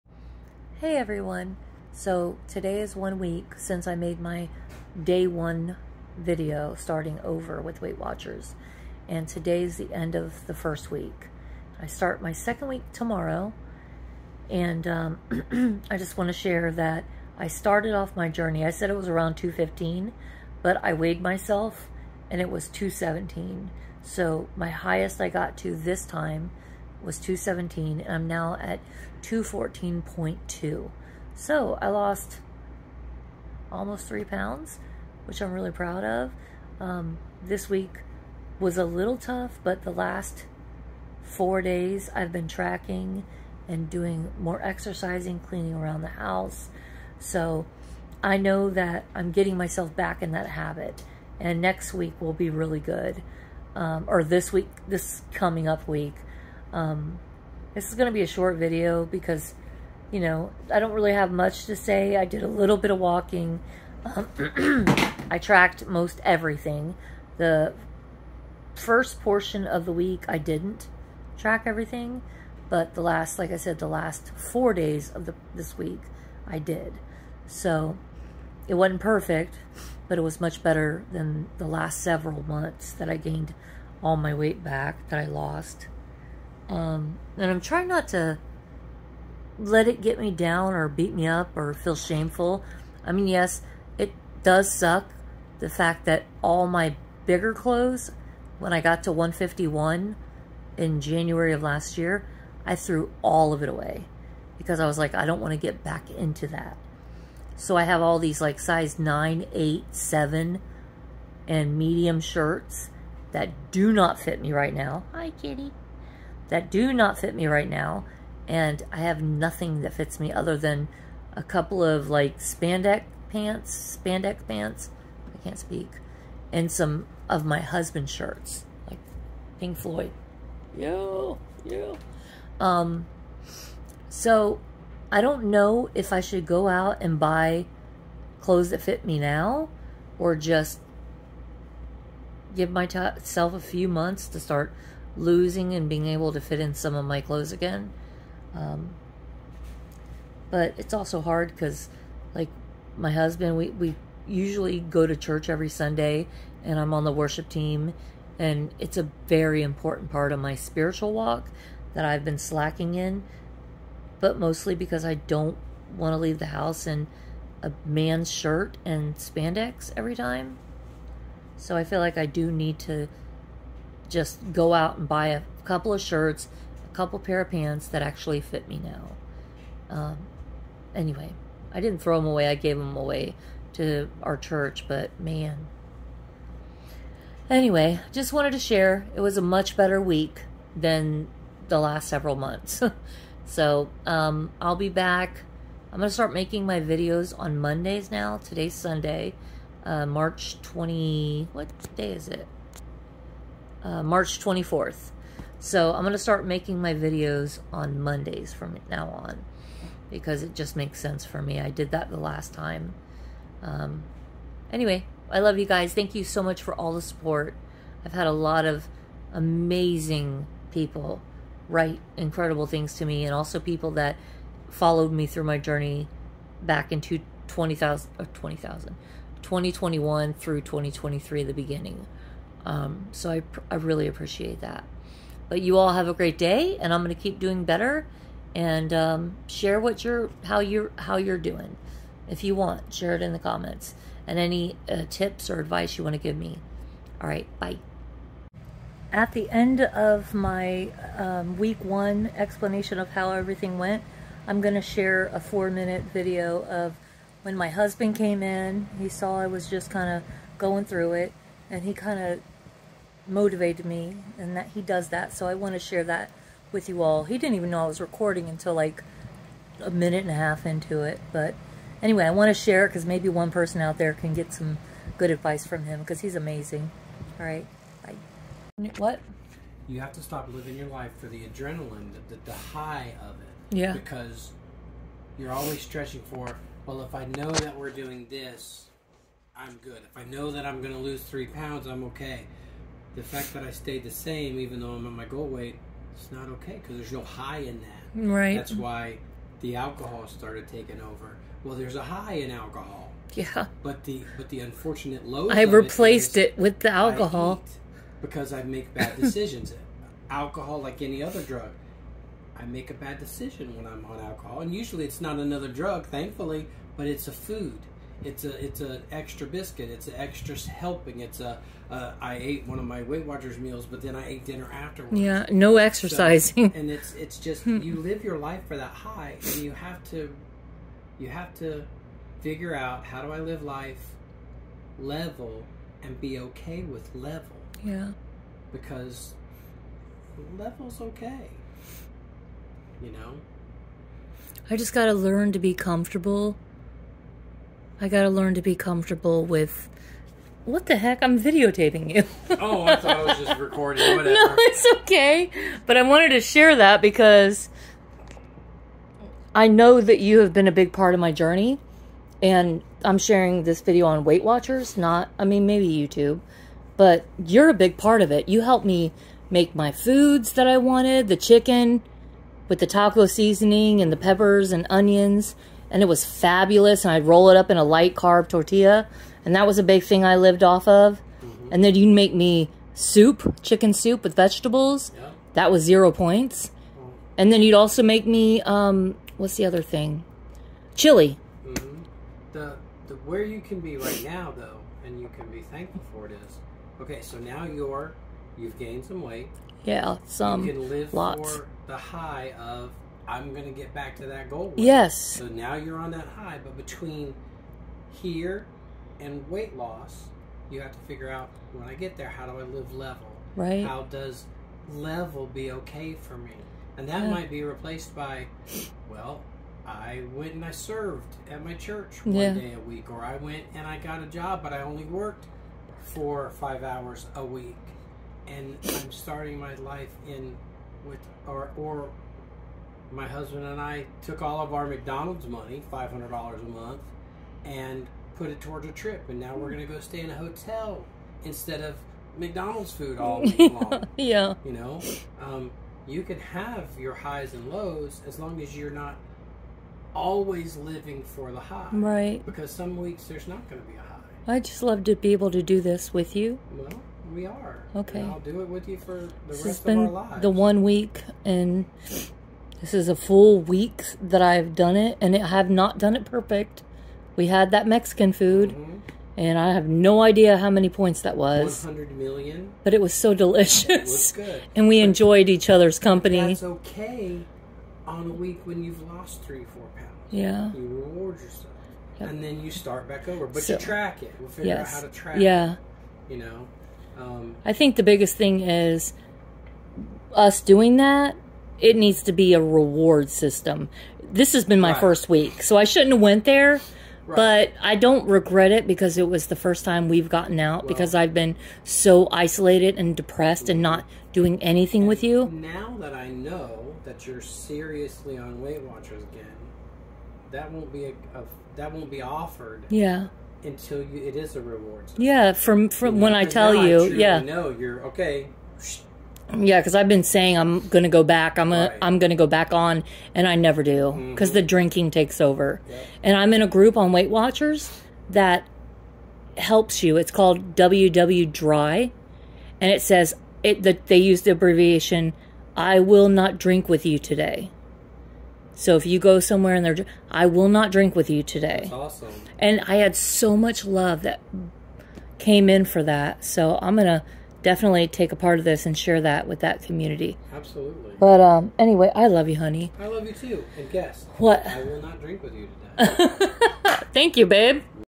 End of Week 1 on Weight Watchers 😊
hey everyone! so this week wasn't perfect but way better than the past several months. I did lose and I'll share it in the video. I also have a special little message for my husband at the end that helped me a lot. I'll see you all on week 2 of Weight Watchers next Monday! I feel very optimistic for the next week! please share if you have any positive stories or you have want any advice. I did lose 86 lb and got within 5 lb of my goal weight so I do know how to do this. I just need to learn how to take my own advice